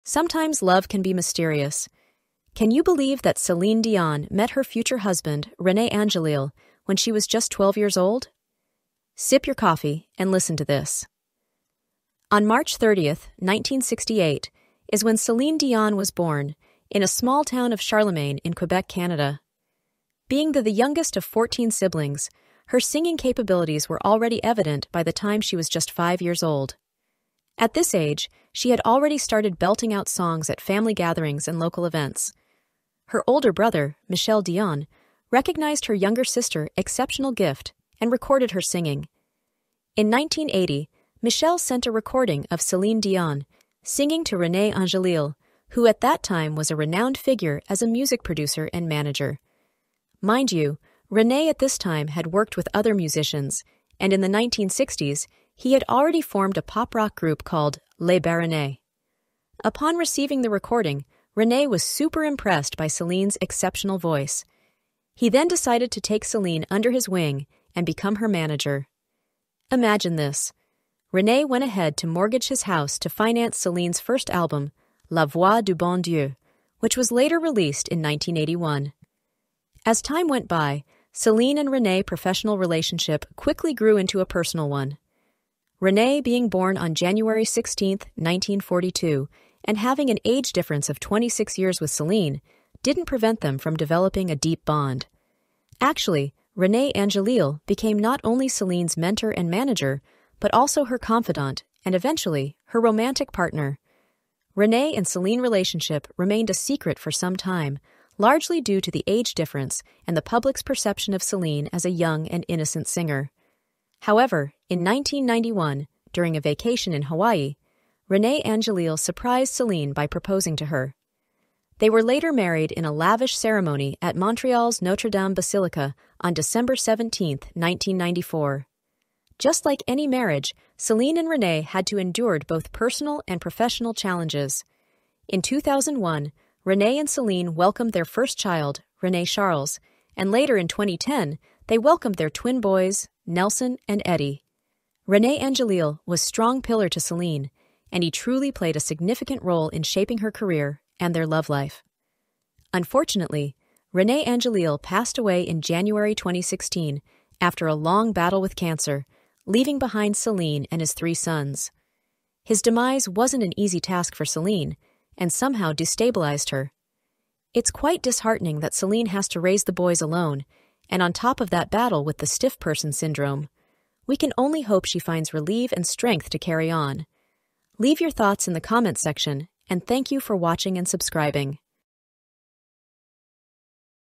How Celine Dion Met Her Late Husband Rene Angelil
Sometimes love can be mysterious. Can you believe that Celine Dion met her future husband, René Angélil, when she was just 12 years old? Sip your coffee, and listen to this. On March 30, 1968, is when Celine Dion was born, in a small town of Charlemagne in Quebec. Being the the youngest of 14 siblings, her singing capabilities were already evident by the time she was just five years old. At this age, she had already started belting out songs at family gatherings and local events. Her older brother, Michel Dion, recognized her younger sister exceptional gift and recorded her singing. Find out more by watching this video and thank so much for your continued support.